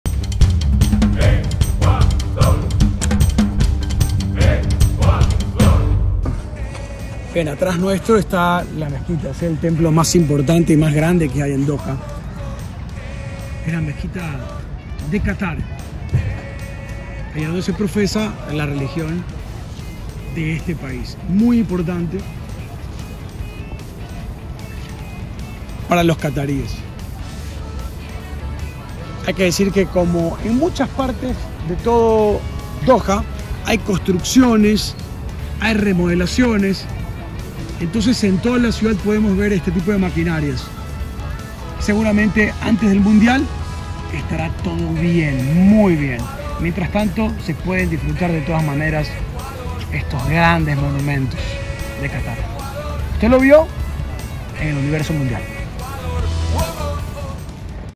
La Mezquita Nacional de Qatar, la más grande del país
La Mezquita Nacional de Qatar se encuentra ubicada en Doha, no muy lejos del Corniche – el paseo marítimo que recorre la bahía de Doha. La mezquita fue construida sobre una pequeña colina para que se pueda ver desde lejos y desde su patio exterior se puede admirar una de las vistas más bonitas de la ciudad.

Francisco Molestina nos cuenta algunos detalles de cómo se prepara esta zona turística previo al mundial de fútbol en noviembre próximo.